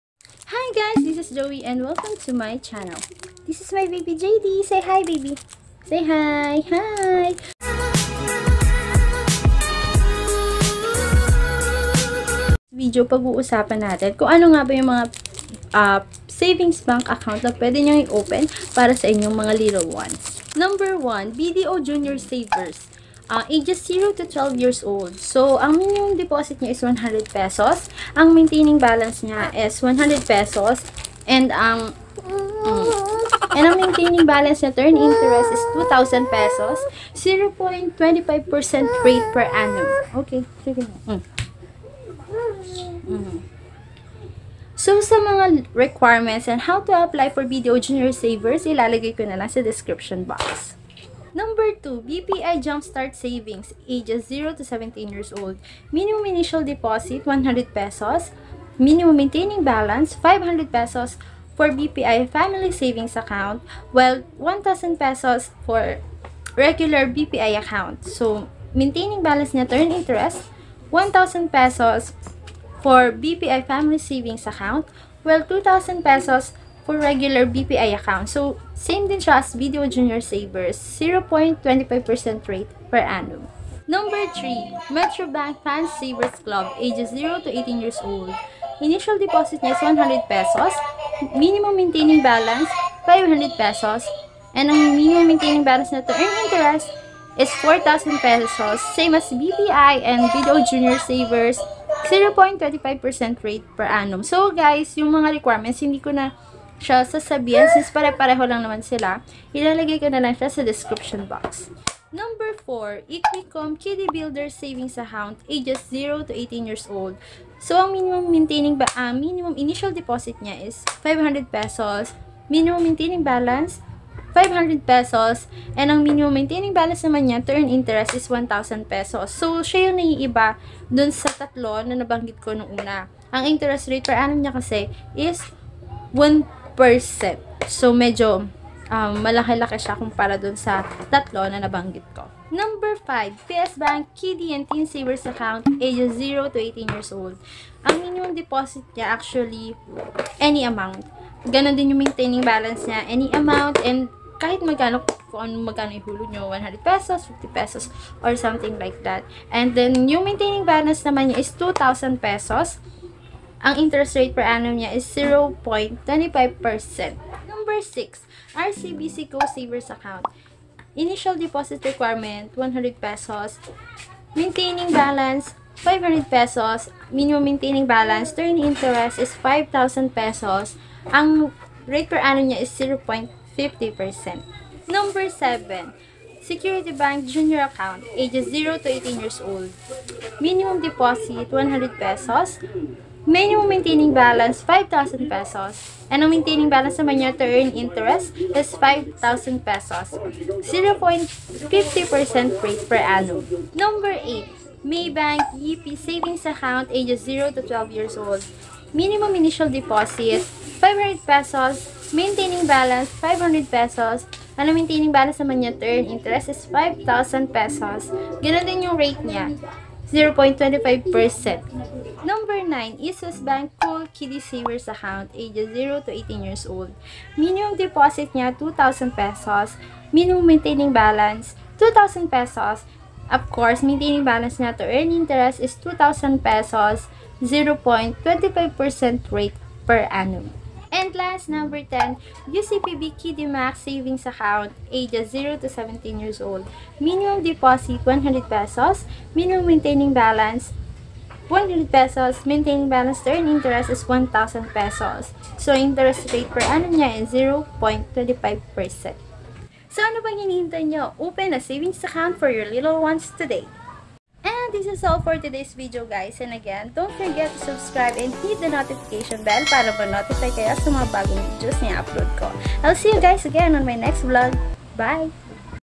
Savings Bank Account na puwede sa bata para sa ipon challenge ni baby
BDO Junior Savers

With an initial deposit of Php2,000.00, kids may opt for an ATM Debit card which also serves as their Junior Savers ID Card.

Kid’s Age Requirement: 7-12 years old

Requirements

Your child’s birth certificate –2 government-issued ID’s1 ID picture 1×1 or 2×2500 pesos minimum

Features

Initial deposit: Php100Minimum monthly ADB: Php100Minimum balance to earn interest: Php2,000Interest rate: 0.25%

BPI Jumpstart Savings
Prepare the following requirements: At least 2 valid IDs. Two pieces recent 1×1 coloured pictures. ...Pay a visit to the nearest BPI branches(or any BPI branches you prefer).Go to New Account Section and tell the banker your intention.Fill-out an application form.

Metrobank Fun Savers Club

Here are the requirements for opening a Fun Savers Account for your kids aged 0 - 17 years old.

1. Visit any nearest Metrobank branch
2. Bring one (1) valid ID*
3. Birth certificate of the child
4. Bring the initial deposit of Php 100.00 only
5. Student ID issued and duly-signed by the principal or head of the school (for kids aged 7 and above)

Equicom Kiddie Builders Savings

Features
Minimum Initial Deposit: Php500Maintaining ADB: Php500Interest Earning Balance: Php1,000Interest Rate: 1.00% p.a.Transaction Record: Passbook

Requirements
In-Trust-For Accounts (ITF) – in behalf of children 0-6 years oldOne (1) Valid ID of the ParentNSO copy of Birth Certificate of the Child – photocopy is required, but original copy shall be required for authenticationPrincipal Accounts – for children 7 to 13 years oldOne (1) Valid ID from the Child.

PSBank Kiddie and Teen Savers
Requirements
Valid IDs

Parents / Legal Guardians:

At least one primary ID or two valid secondary IDs, one of which with photo and signature

Child/ren or Ward/s

Birth Certificate issued by the NSO or Certified True Copy issued by the Local Civil RegistrarPassport or School ID (photo bearing duly signed by the principal or head of the school)

Note: Kids / teens from 7-17 years old should be accompanied by his parent(s) or legal guardian/s during account opening.

RCBC GO Savings account
Requirements

Valid Id
Latest Baranggay Certificate ng iyong anak
Birthcertificate
Id Picture

SECURITY BANK JUNIOR ACCOUNT
Requirements
One (1) Valid ID of the Parent
NSO copy of Birth Certificate of the Child – photocopy is required, but original copy shall be required for authentication.

MAYBANK YIPPIE SAVINGS ACCOUNT
Requirements
Child: Original birth certificate and MyKid
Parents / Guardian: Original Identity Card (IC) for Malaysians andPassports for foreigners. For passport, other supporting documents that may be required are valid working/student permit, valid visa, letter of confirmation from Employer/Education Institutions (whichever is relevant)

Eastwest Bank Cool Savers Kiddie Account
Your child’s birth certificate
–2 government-issued ID’s
1 ID picture 1×1 or 2×2

UCPB Kiddie Max savings account
Requirements
In-Trust-For Accounts (ITF) – in behalf of children
0-6 years old
Principal Accounts – for children and teens 7 to 17 years oldOne (1) Valid ID from the Child